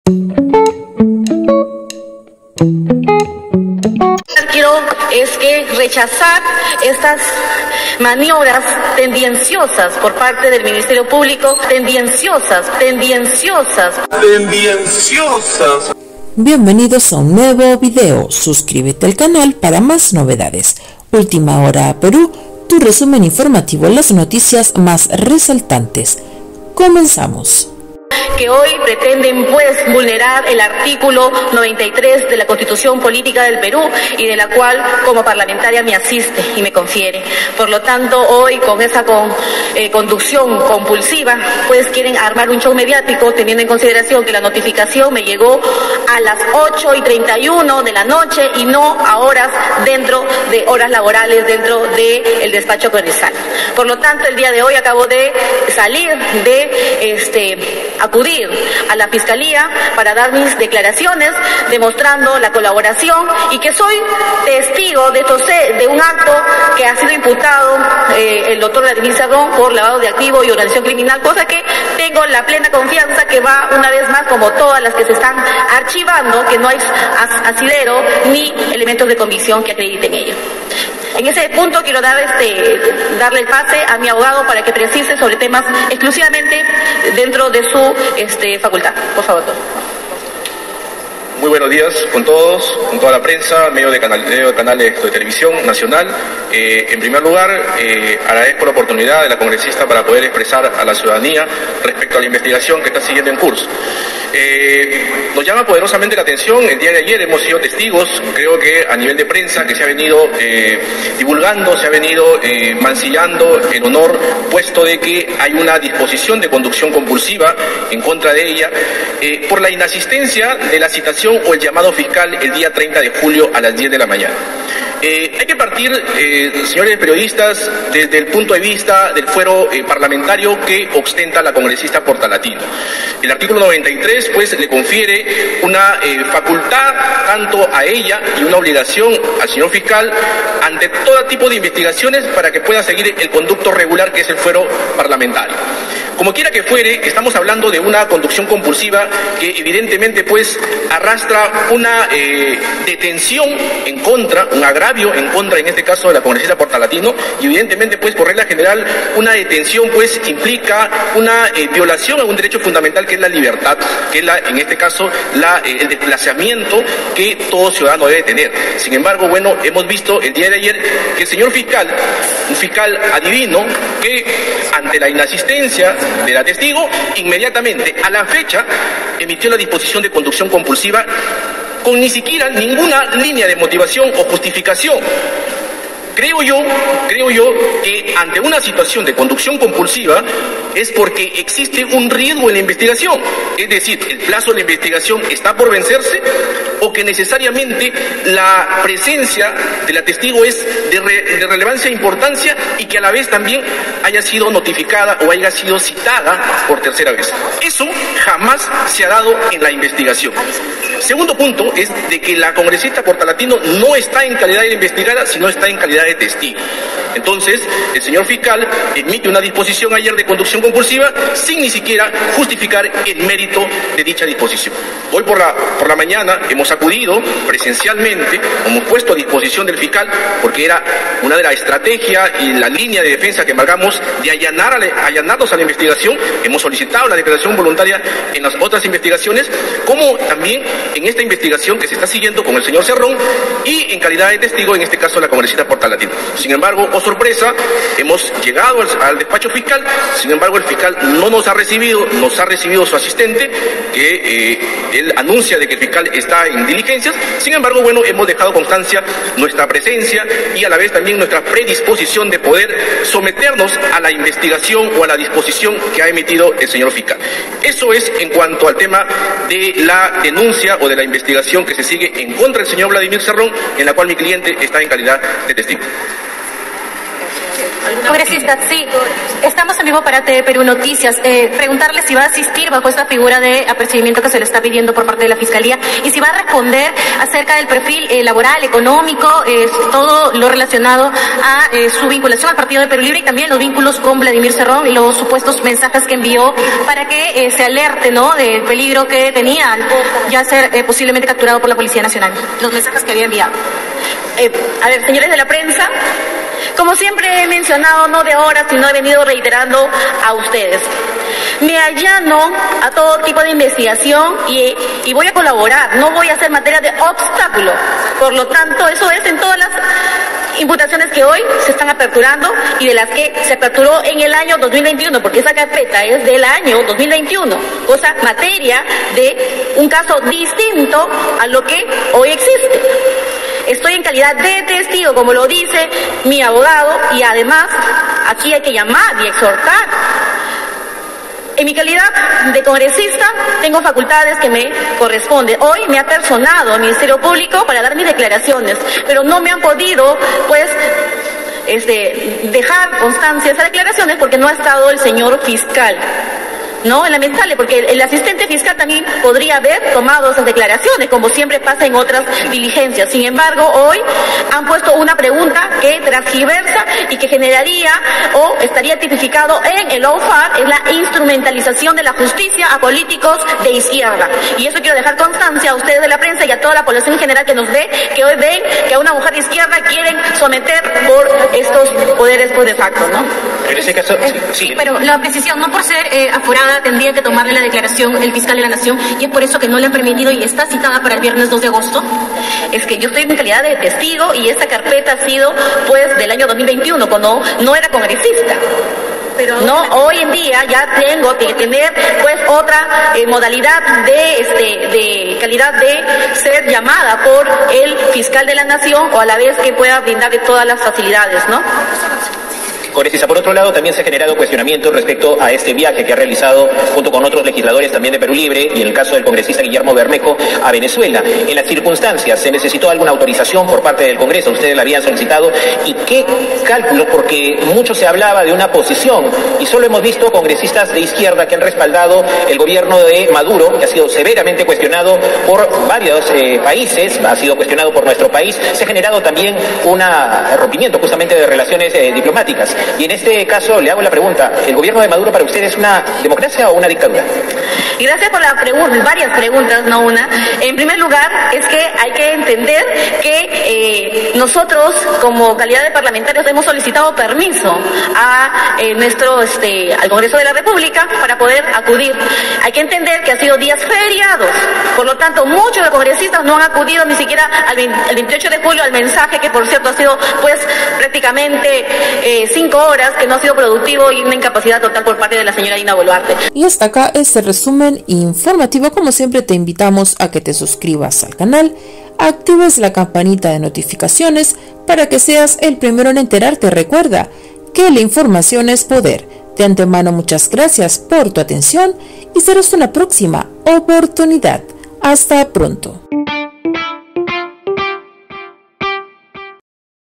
LE DAN DURO!! FISCALIA LE DA REVOLCON A KELLY PORTALATINO POR CHATS CON VLADIMIR CERRON Y PERÚ LIBRE
MUSICA SIN COPYRIGHT PARA TUS VLOGS Y VIDEOS EN GENERAL EN                    @DJPileusmusic